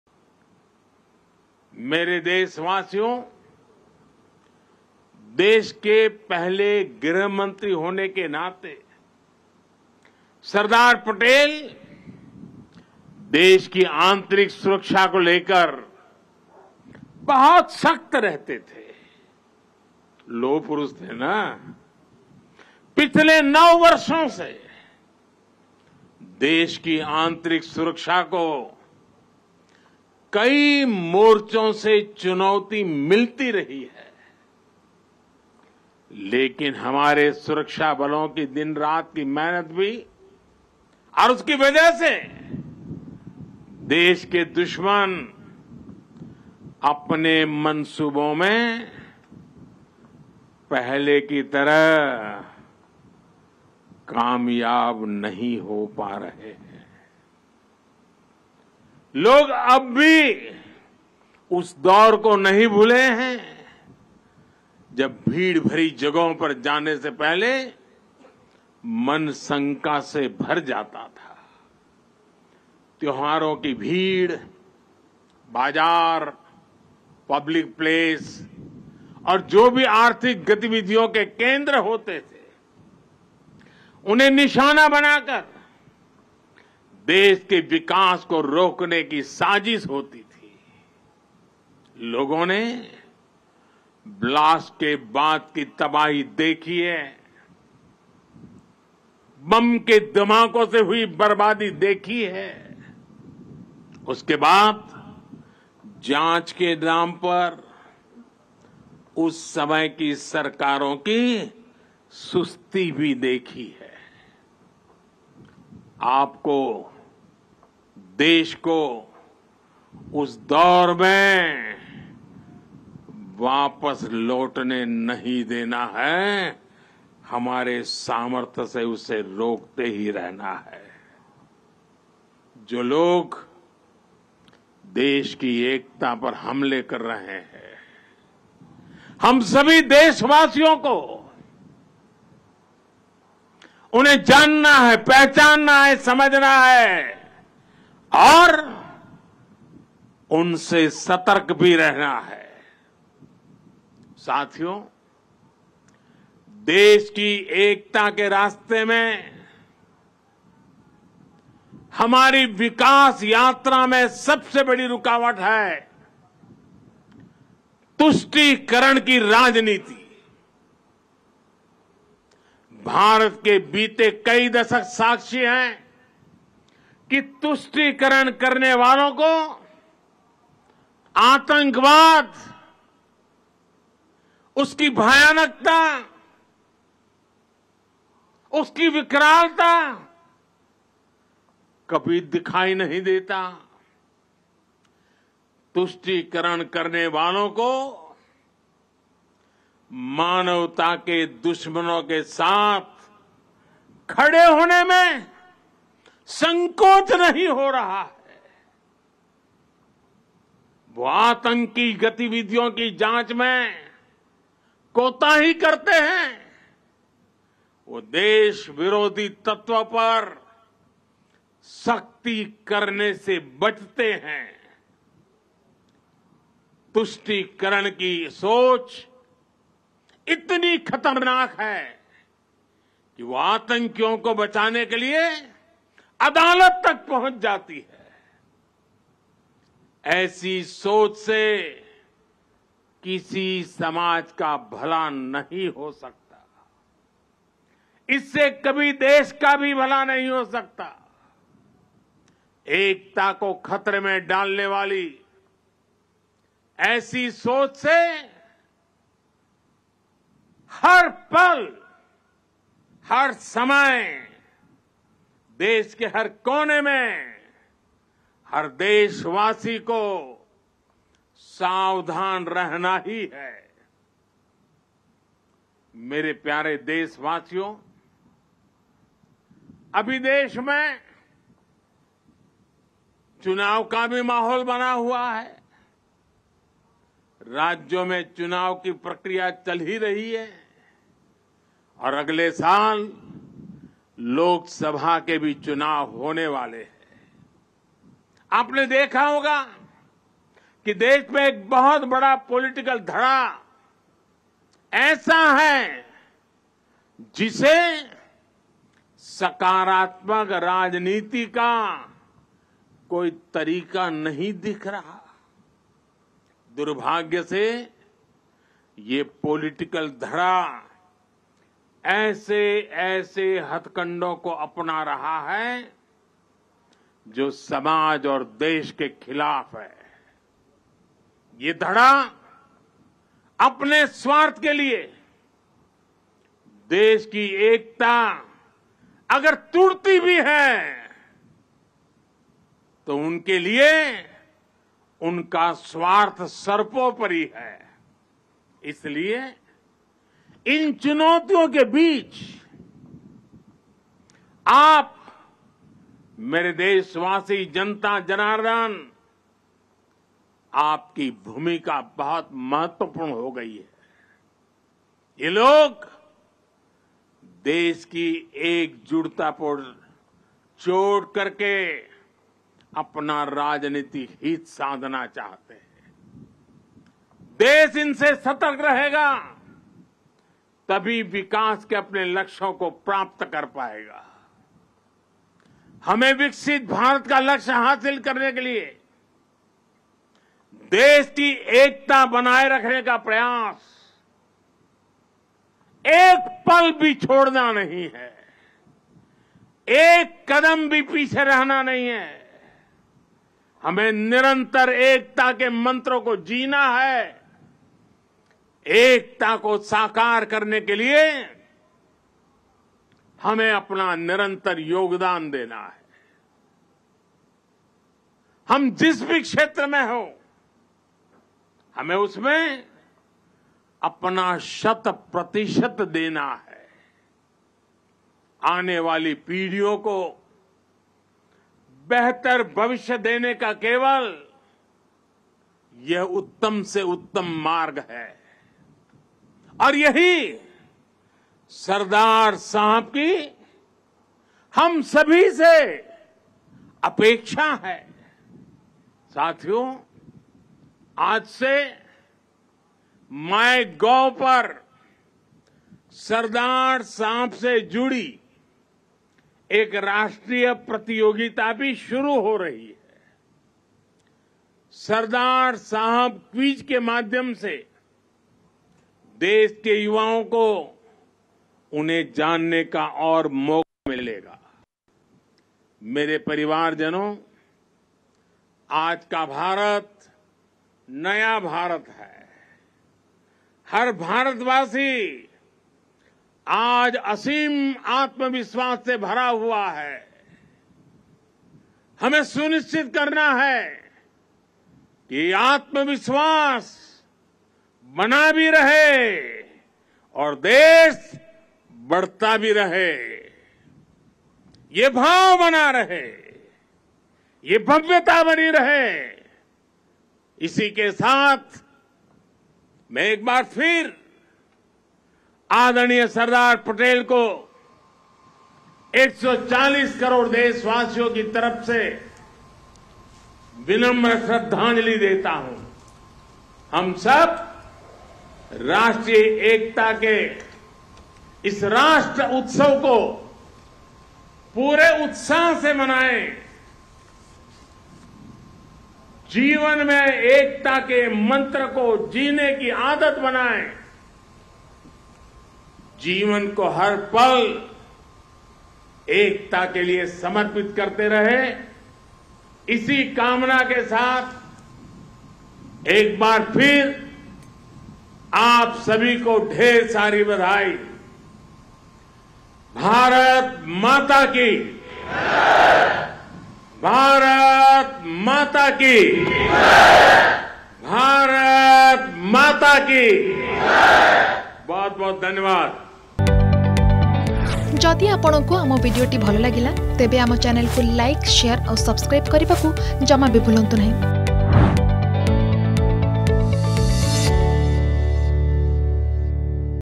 ସର୍ଦ୍ଦାର ପଟେଲ ଦେଶର ଆନ୍ତରିକ ସୁରକ୍ଷାକୁ ନେଇ ବହୁତ ଶକ୍ତ ରହୁଥିଲେ || Narendra Modi || National Unity Day
ସର୍ଦ୍ଦାର ପଟେଲ ଦେଶର ଆନ୍ତରିକ ସୁରକ୍ଷାକୁ ନେଇ ବହୁତ ଶକ୍ତ ରହୁଥିଲେ: ପ୍ରଧାନମନ୍ତ୍ରୀ ନରେନ୍ଦ୍ର ମୋଦି. ।